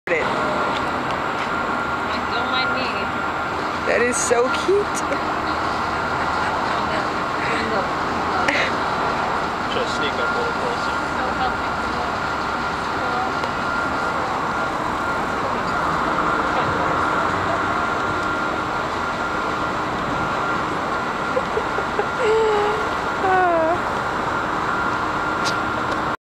Squirrel Eating A Cheeseburger
I was walking to class one day and came across this cuteness!